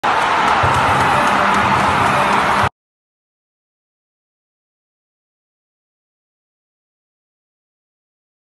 ERICK AYBAR NUNCA PISO HOME ?????..... LICEY
LIDOM🇩🇴 Están seguros todos los fanáticos que Erick Aybar toco el homeplate ? bueno ya es tarde para el reclamo esa carrera los llevo a la Serie Final (2019-20)..un ejemplo perfecto para todos los jugadores de béisbol, siempre asegúrate que el jugador contrario toque la base o el plato, en esta el receptor nunca miro